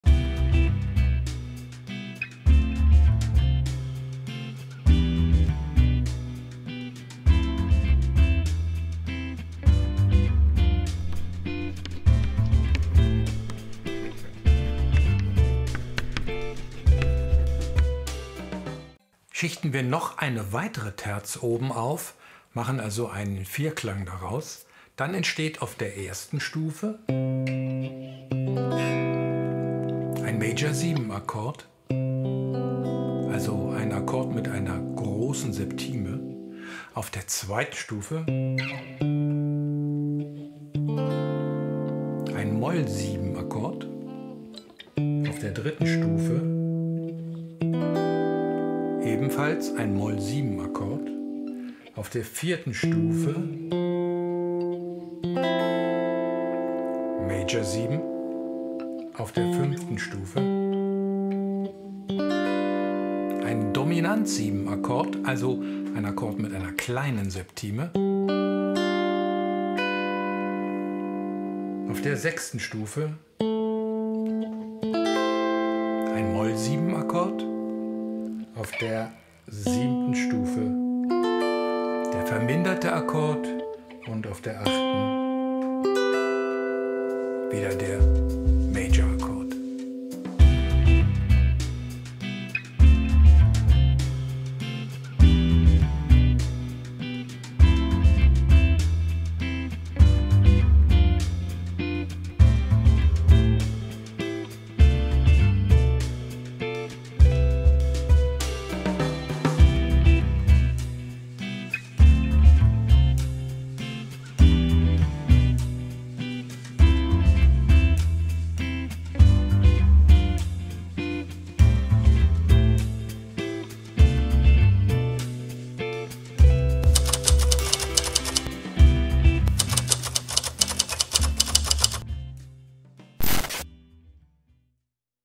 Eine Terz on Top - Diatonische Vierklänge der Dur-Tonleiter.
Schichtest Du noch eine weitere Terz auf unsere Dreiklänge, dann erhältst Du die diatonischen Vierklänge einer Dur Tonleiter. Unzählige Songs bestehen allein aus Kombinationen dieser sieben Akkorde. Schreibe Deine eigenen Hits und kombiniere diese Akkorde so wie es Dir gefällt.

Dies ist nur ein Ausschnitt aus dem Video „Basic 02 - Die Entstehung der diatonischen Stufenakkorde“. Das komplette Video findest Du auf YouTube oder der Website meines Video-Blogs Von dort kannst Du auch alle Akkorddiagramme und den Backing-Track frei downloaden.